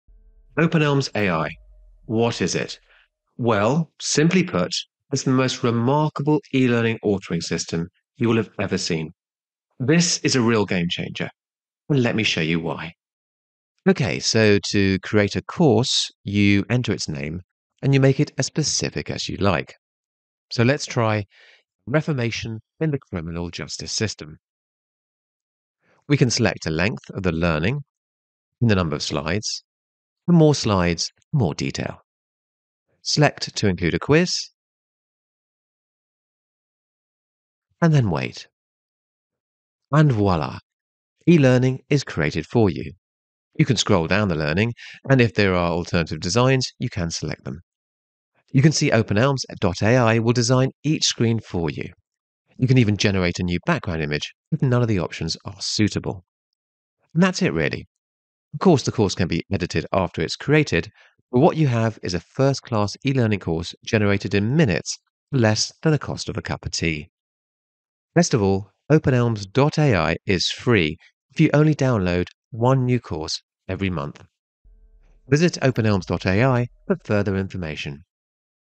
Open eLMS AI - Create eLearning in 60 seconds
and register to be notified as soon as the openelms.ai free version is launched (end of April 2023).

openelms.ai is an elearning creation system which delivers learning courses of breathtaking quality from a single line of text.

This Adobe Captive / Articulate killer is a read game-changer for education and corporate training.

Chapters:
0:00 Introduction
0:13 openelms.ai demo